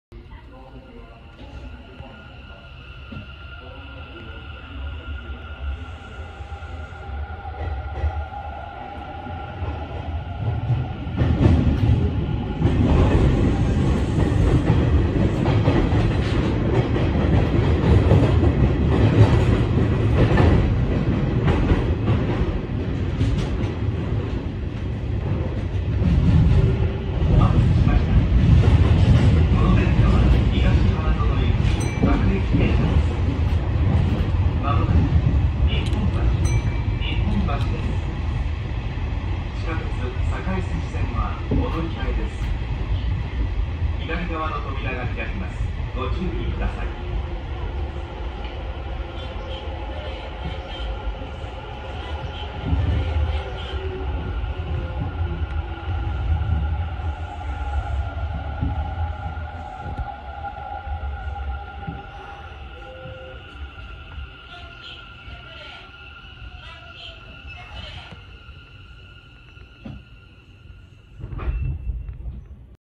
[前面展望]近鉄難波線大阪難波駅から近鉄日本橋駅
近鉄難波線大阪難波駅は、難波線の起点であり、奈良線の列車は全て難波線に直通するため奈良線における大阪側のターミナルとなっている。奈良線方面に直通する列車は特急から普通まで全ての種別が発着するが、大阪線方面に直通する列車は名古屋・伊勢志摩方面に向かう一部の特急のみとなっており、特急以外の列車は当駅には入線せず、2駅隣の大阪上本町駅地上ホームから発着している。
島式ホーム1面・単式ホーム1面による2面3線の地下駅で、南北に貫くOsaka Metro御堂筋線・四つ橋線の下を通るために、東西に並行する千日前線ホームと同じ地下3階にある。改札口は東西2か所に設けられている。桜川側に引き上げ線が1本あり、近鉄難波線方面からの折り返しに用いられている。
桜川側の引き上げ線は1線だが、当駅発着の列車は多数設定されている。そのため、特急列車はメイン乗り場をホーム前寄り（大阪上本町側）にし、後ろ寄りの空いたスペースに次の列車をいち早く待機させる縦列停車の手法を採ることもある。なお、かつて3本あった引き上げ線のうち2本は、西九条駅から延伸された阪神なんば線の本線線路に転用されたため、代わりに桜川駅のドーム前側に新しく2本の引き上げ線が設置され、回送列車は桜川駅で折り返す形態がとられるようになった。そのため、当駅での乗務員交代は行わず、近鉄の乗務員が桜川駅まで担当する。